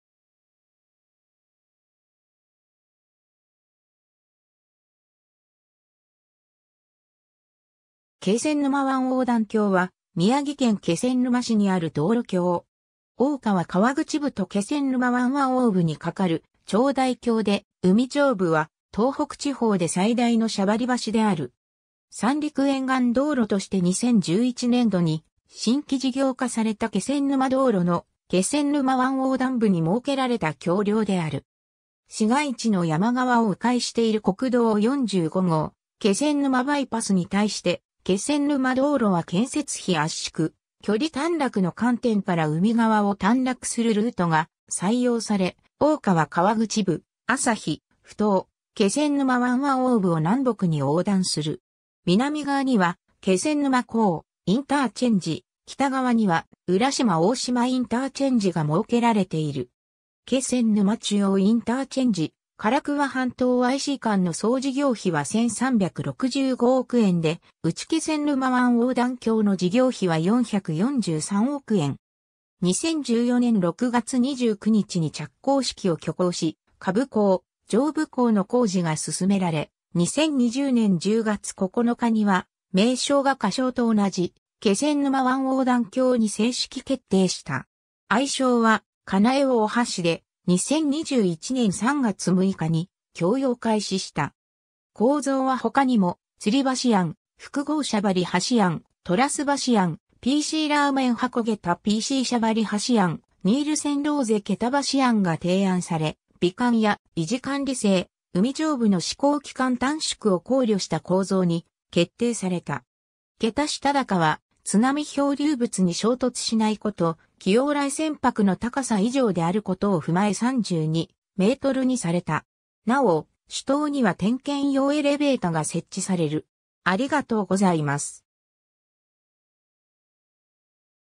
気仙沼湾横断橋
気仙沼湾横断橋（けせんぬまわんおうだんきょう）は、宮城県気仙沼市にある道路橋。
大川河口部と気仙沼湾湾奥部に架かる長大橋で、海上部は東北地方で最大の斜張橋である。
 三陸沿岸道路（三陸縦貫自動車道）として2011年（平成23年）度に新規事業化された気仙沼道路の気仙沼湾横断部に設けられた橋梁である。
市街地の山側を迂回している国道45号気仙沼バイパスに対して、気仙沼道路は建設費圧縮、距離短絡の観点から海側を短絡するルートが採用され、大川河口部、朝日埠頭、気仙沼湾湾奥部を南北に横断する。
南側には気仙沼港インターチェンジ、北側には浦島大島インターチェンジが設けられている。
気仙沼中央IC - 唐桑半島IC間の総事業費は1365億円で、うち気仙沼湾横断橋の事業費は443億円。
2014年（平成26年）6月29日に着工式を挙行し、下部工・上部工の工事が進められ、2020年（令和2年）10月9日には名称が仮称と同じ「気仙沼湾横断橋」に正式決定した。
愛称は「かなえおおはし」で、2021年（令和3年）3月6日に供用開始した。
 構造は他にも、吊橋案、複合斜張橋案、トラス橋案、PCラーメン箱桁+PC斜張橋案、ニールセンローゼ桁橋案が提案され、美観や維持管理性、海上部の施工期間短縮を考慮した構造に決定された。
桁下高は津波漂流物に衝突しないこと、既往来船舶の高さ以上であることを踏まえ32 mにされた。
なお、主塔には点検用エレベータが設置される。